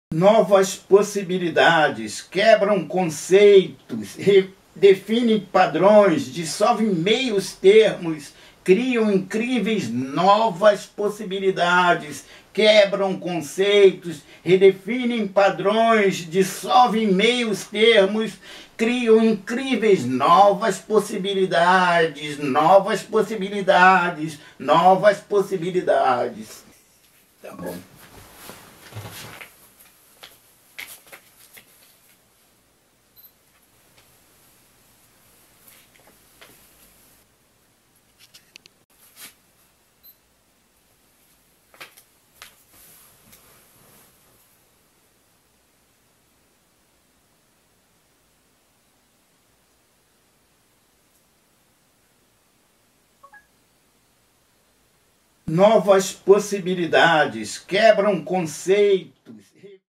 Moduan Matus em Novas Possibilidades
Poesia de Moduan Matus publicada na edição 16 da Gambiarra Profana.